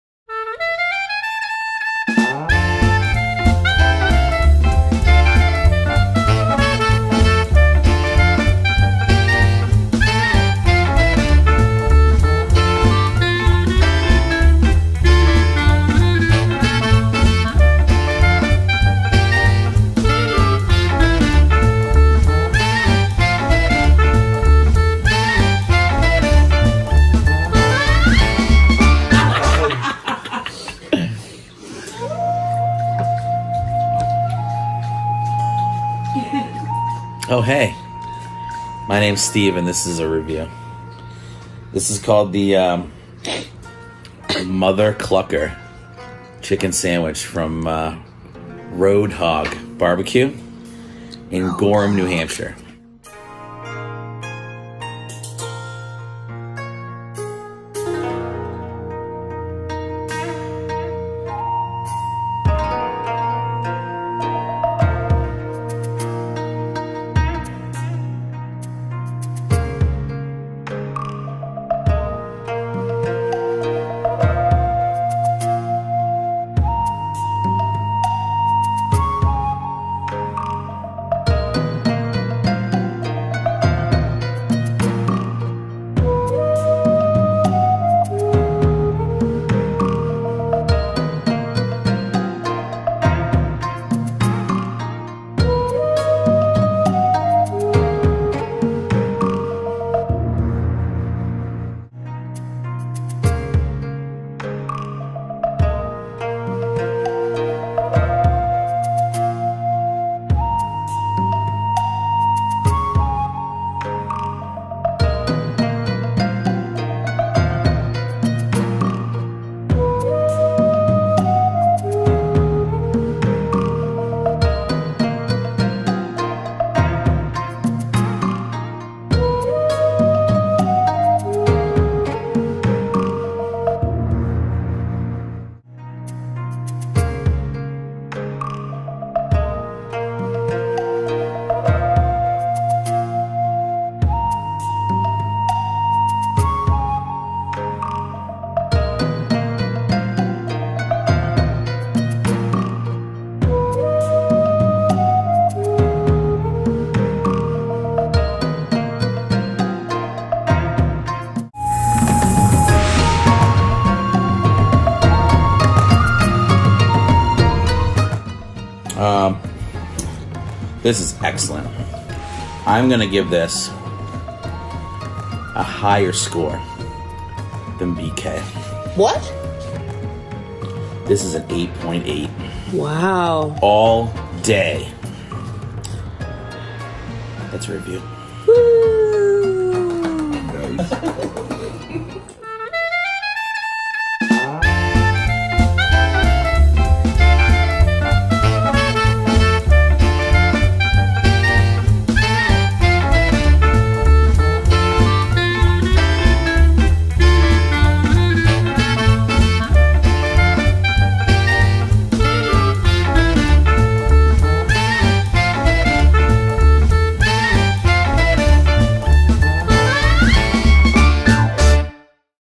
Steve Reviews: Road Hawg's Mother Cluckin' Chicken Sandwich
Steve takes his review show on the road up to Gorham, New Hampshire to try the world famous "Mother Clucker Chicken Sandwich" from Road Hawg BBQ. Click the review to see if this sandwich holds the keys to the door that leads you to Chicken Sandwich Valhalla.